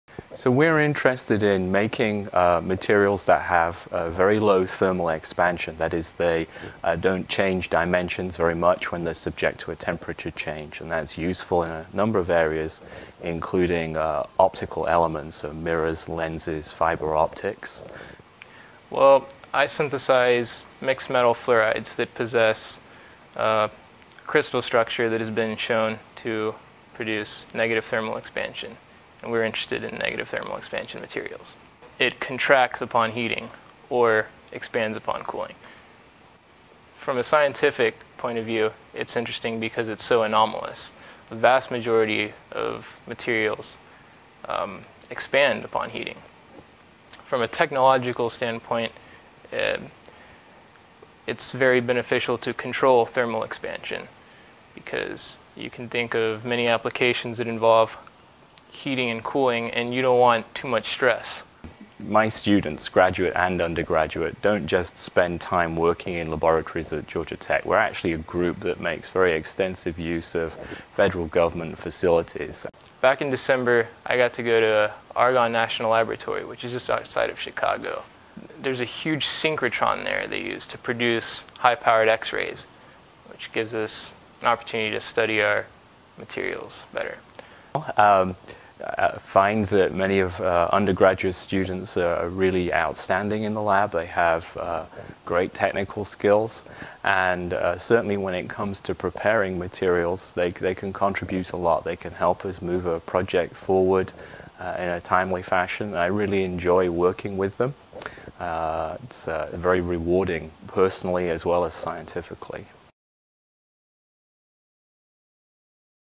Investigating Thermal Expansion
Angus Wilkinson, chemistry professor, details his work with undergraduate students, like Justin Hancock, in his lab as they investigate the properties of thermal expansion.

Video: Drew Schneider, David Terraso, Khalen Morehead